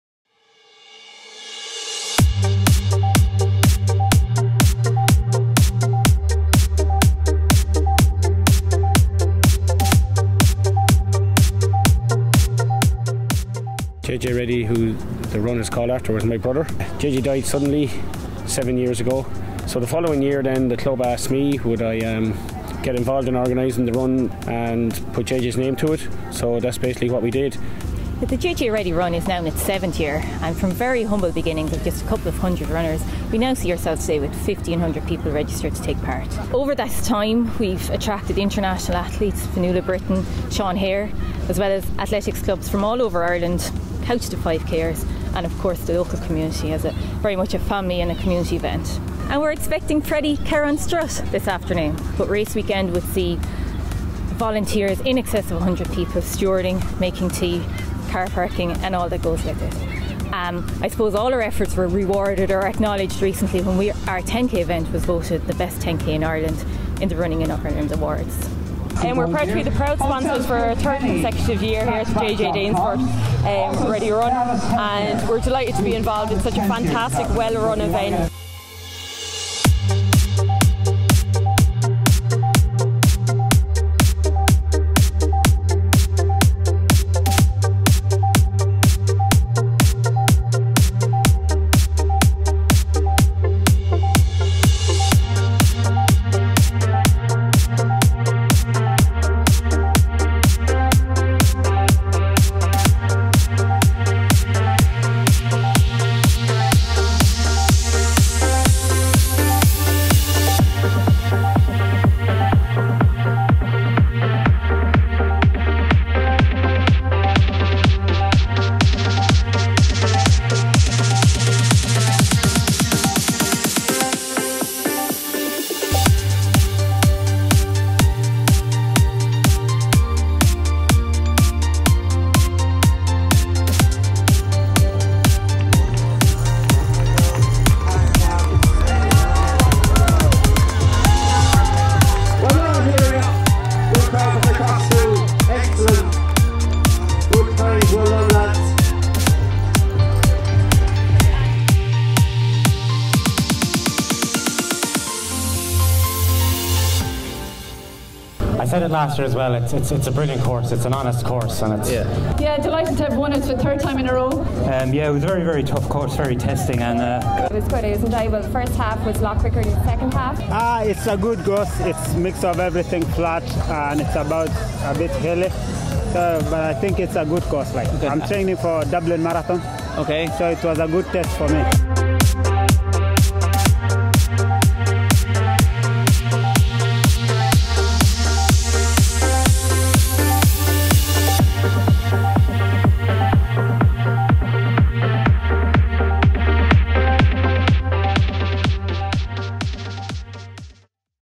Danesfort Athletes
Awarded the Best Road Race in Ireland 2017 (as voted by our participants via the Running In Ireland Awards) the JJ Reddy Danesfort Run Kilkenny caters for all from pro's, to novice, right down to the fun runners. From very humble beginnings, the award winning JJ Reddy Danesfort Run is now in it's eight year (2019) and is Kilkenny's largest road running event. Incorporating a Half Marathon, 10k, 5k and family fun run there is something to suit all ages and abilities.  Attracting athletes and their family & friends from all four corner of Ireland, and even beyond, a fun festival-style event is guaranteed. With water stations and stewards throughout the course, marked in both KM's & miles, experienced pacers, chipped timing, on-site DJ, a range of exhibitors, along with convenient & secure on site parking and complimentary post race refreshments the JJ Reddy Danesfort Run is a firm fixture on the running calendar for those wanting value and quality for their entry fee. See you on Sept 15th 2019 !!!